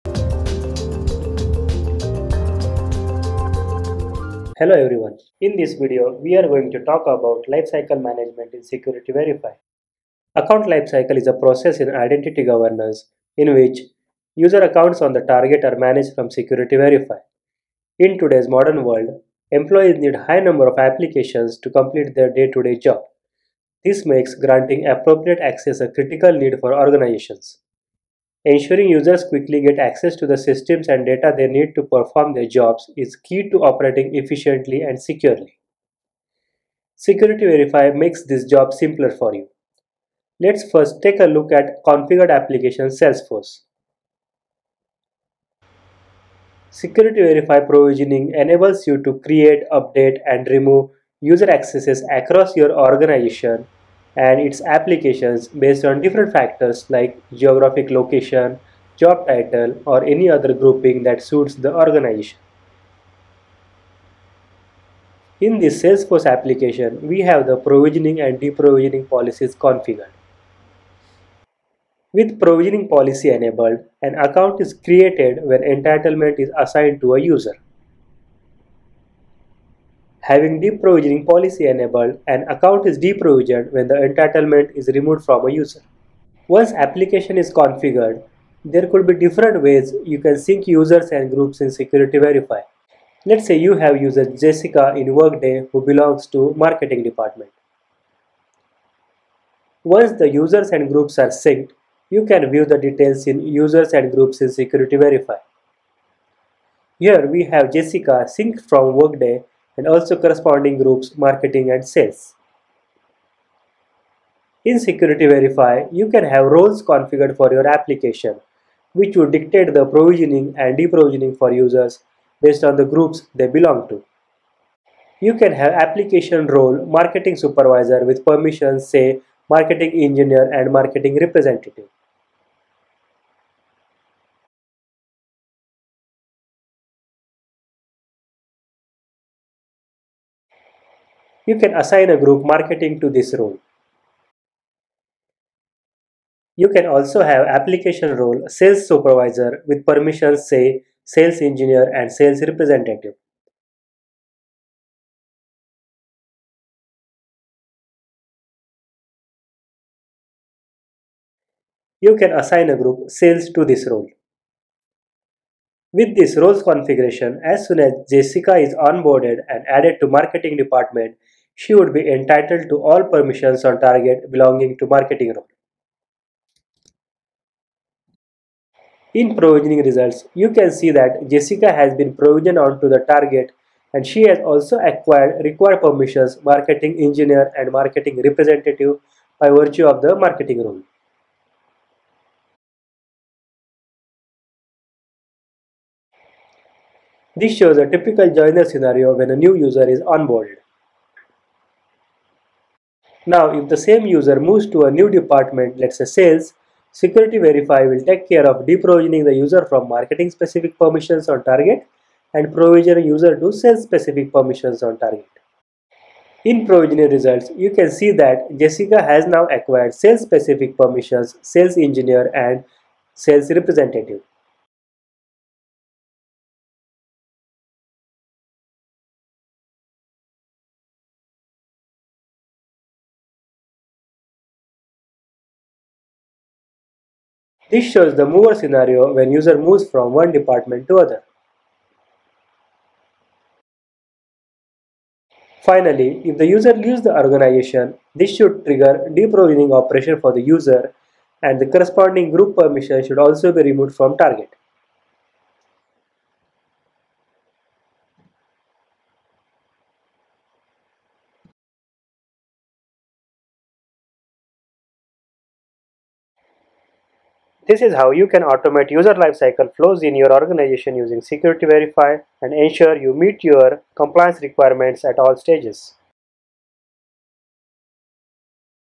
IBM Security Verify: Account lifecycle management
In this video IBM Support presents how you can use IBM Verify to manage your user's account lifecycles.

Creator: Tushar and Aditya